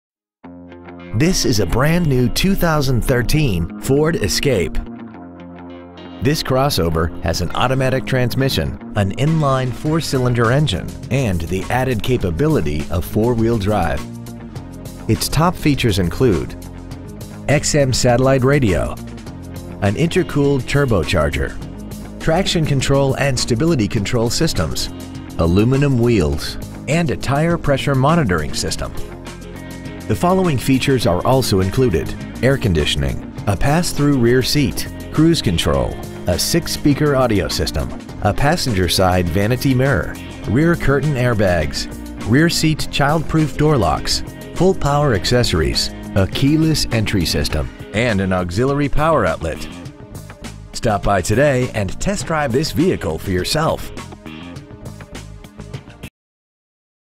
2013 FORD ESCAPE Hackensack NJ
We have been honored to serve the Hackensack NJ area , we promise that your experience at our dealership will exceed your expectations ! Year : 2013 Make : FORD Model : ESCAPE Engine : 1.6L I4 16V GDI DOHC TURBO Trans . : 6-SPEED AUTOMATIC Exterior : TUXEDO BLACK Miles : 16 Interior : CHARCOAL BLACK Stock : 13T2088 All American Ford Inc. River Street Hackensack , NJ 07601